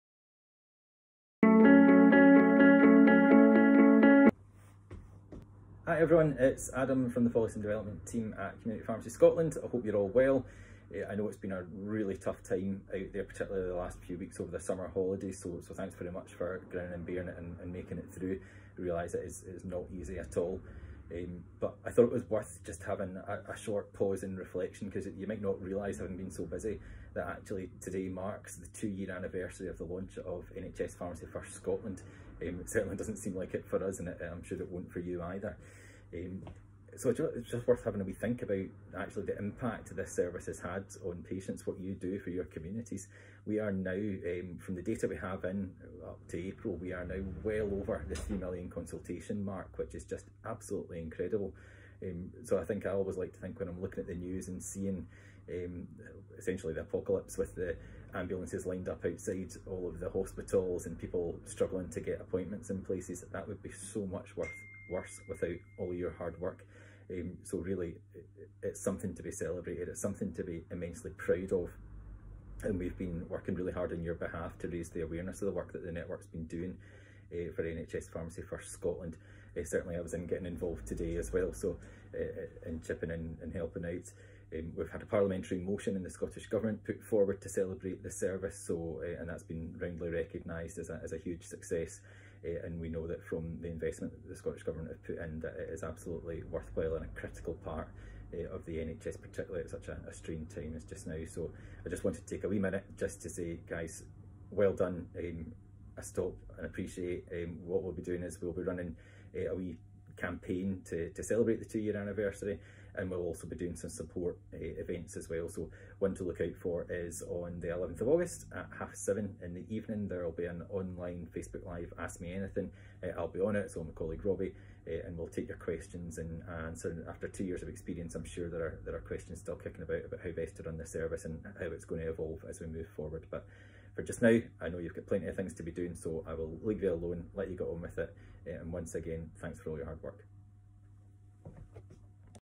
NHS Pharmacy First Scotland - 2nd Anniversary
With the 2-year anniversary of NHS Pharmacy First Scotland fast approaching we wanted to take this moment to reflect on the service and what it has achieved for the people of Scotland.